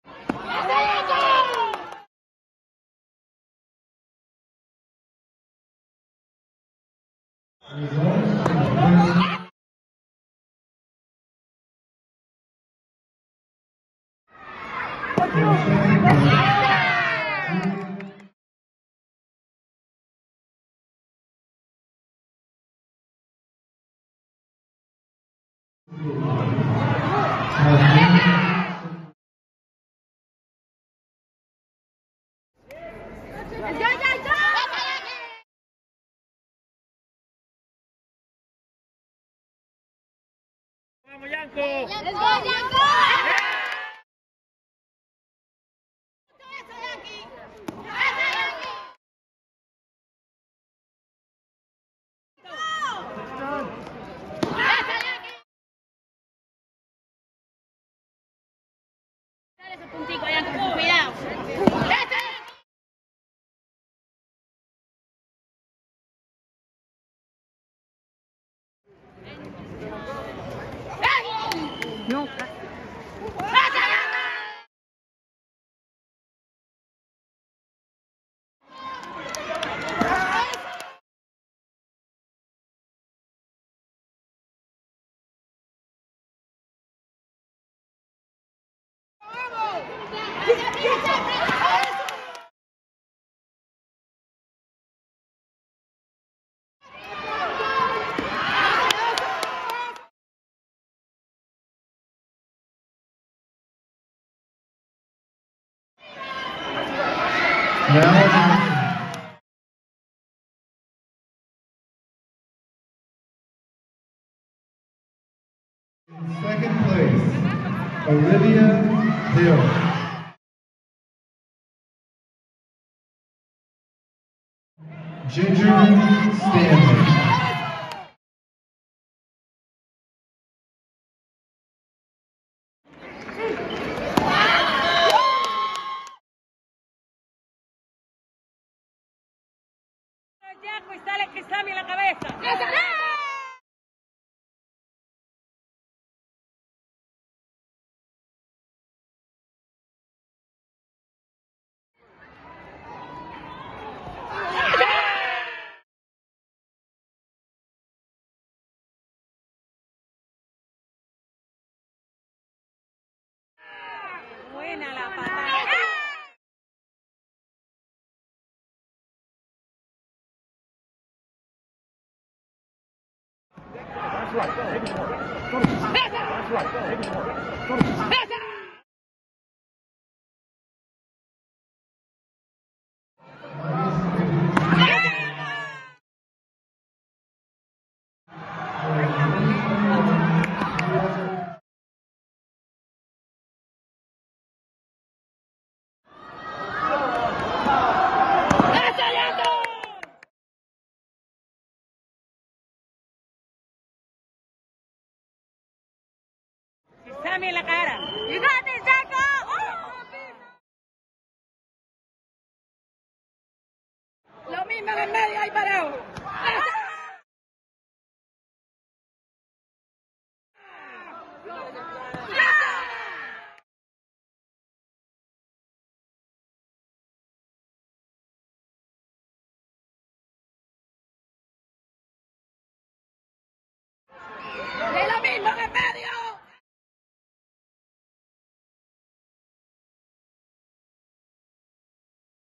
WKF Karate USA Open VEGAS 2021
Janco Dorta gold medal - 61 Kg at Junior International Cup and gold medal in USA Open Vegas 2021